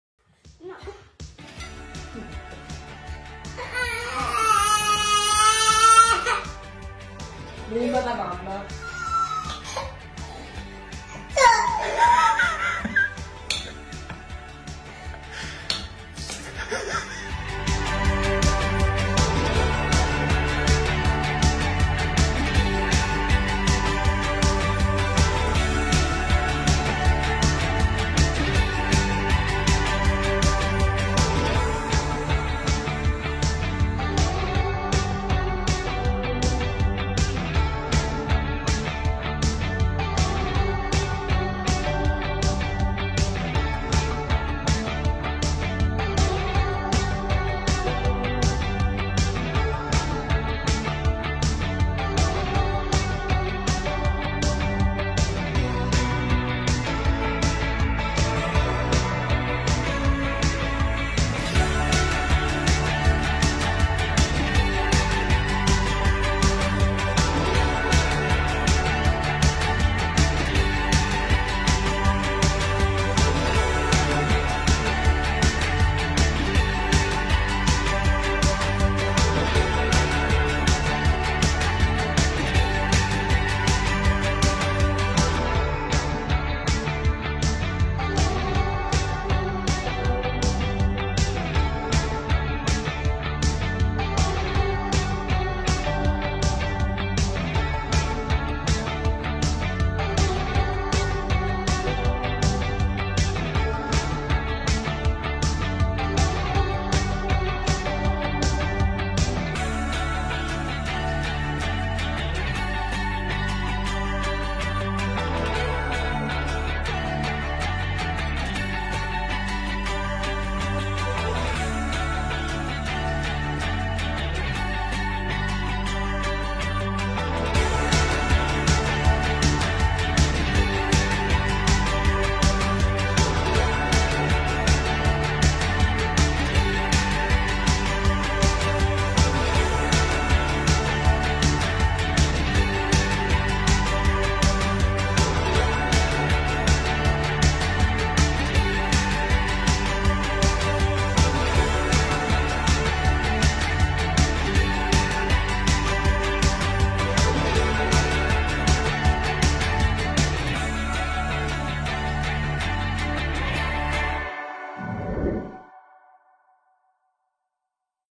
GNOCCHI RICOTTA E SPINACI - Amici in Cucina
GNOCCHI RICOTTA E SPINACI - Video postato da Miciemira Ricette

A parte il momento di protesta con lancio del cappello e tentativo di smontaggio della "scenografia", poi è filato tutto liscio almeno fino alla cottura, complice l'uscita di scena!

Guardate il Video e Mettete MI PIACE al VIDEO